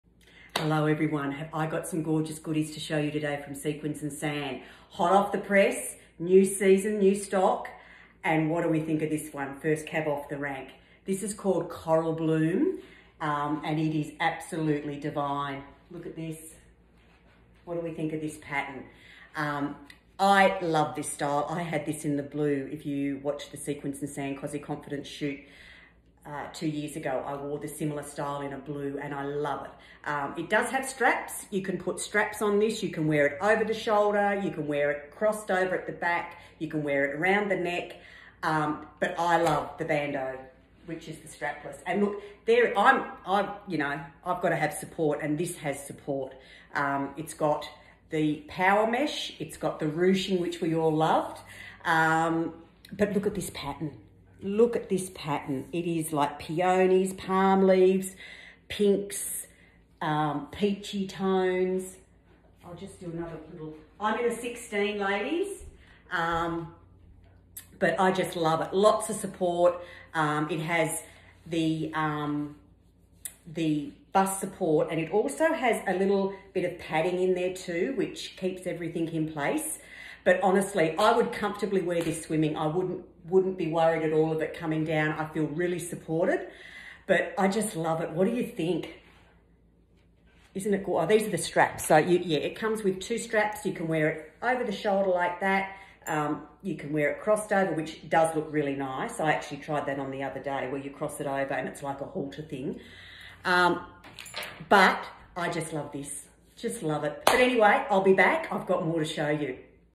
Coral Bloom Bandeau One Piece - In Plus Sizes too.
In this video Maz talks about this swimsuit, in her words, not ours. It's always honest with Maz. Raw and real, just the way it should be.
Thanks!
Anita
@ Sequins and Sand